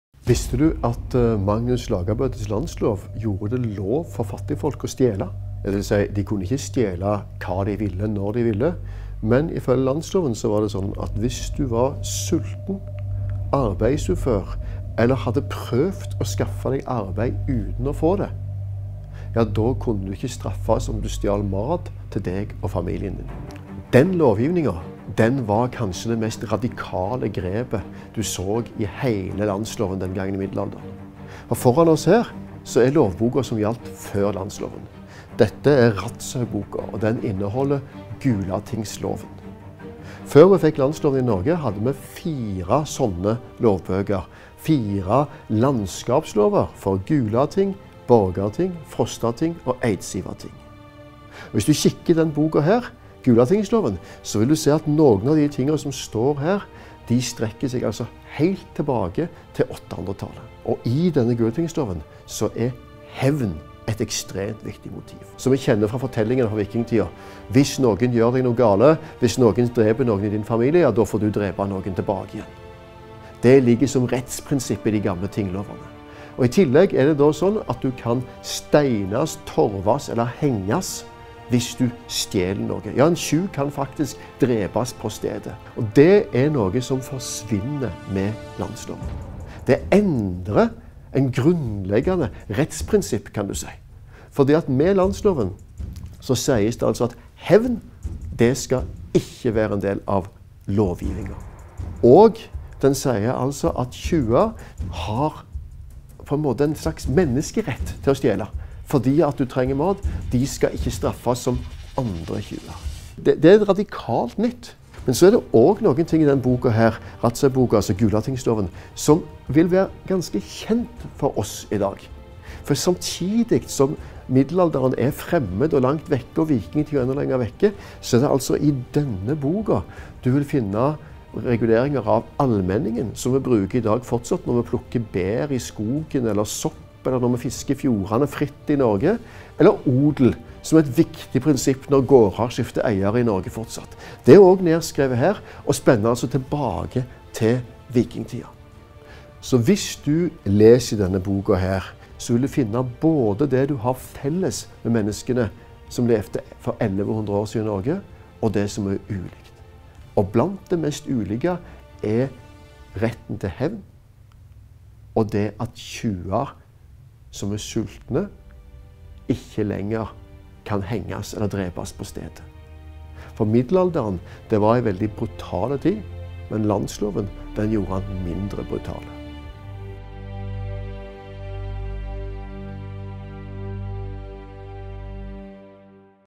Landslova gjorde det lov for fattigfolk å stele
– Kanskje det mest radikale grepet i heile Landslova.

Magnus Lagabøtes landslov kan på mange måtar sjåast som ein overgang frå ei brutal tid prega av hemn og barbari til eit samfunn basert på meir humane verdiar. Var du arbeidsufør og svolten, skulle du ikkje straffast om du stal mat, ifølge Landslova.

Men korleis var lovene før Magnus Lagabøtes landslov?

Nasjonalbibliotekar Aslak Sira Myhre tar for seg nokre av dokumenta frå utstillinga «Med lova i hand. Magnus Lagabøtes landslov, 1274–2024». Av dei gamle lovbøkene som gjaldt i Noreg før Landslova blei innført i 1274, er dette den einaste som fullt ut har overlevd: eit eksemplar av den eldre Gulatingslova. Eigaren er Det Kgl. Bibliotek i København.